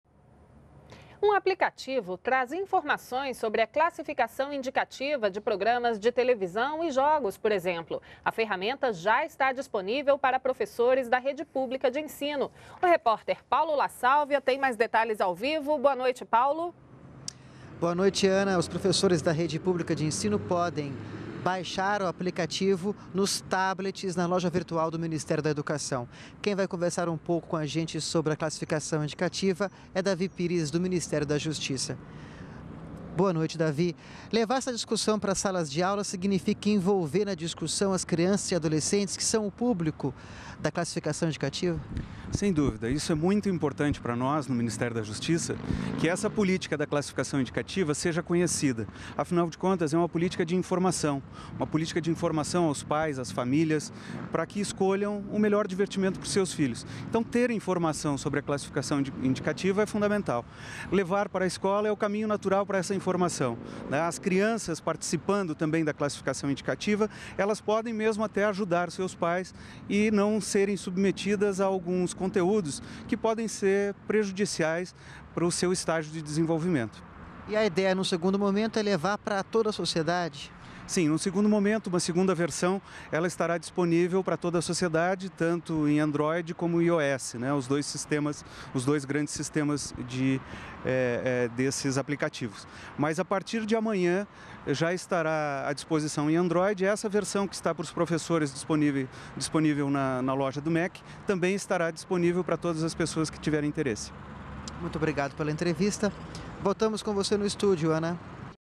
Aplicativo com classificações indicativas auxilia professores da rede pública de ensino
NBR NOTÍCIAS - 07.08.14: Os professores da rede pública de ensino podem baixar o aplicativo que busca
informações sobre a classificação indicativa de programas de televisão, filmes e games por meio da loja virtual do Ministério da Educação (MEC). Segundo o diretor do Departamento de Justiça do Ministério da Justiça, David Pires, a segunda versão do aplicativo estará disponível para a sociedade, em formatos para os sistemas operacionais Android e IOS.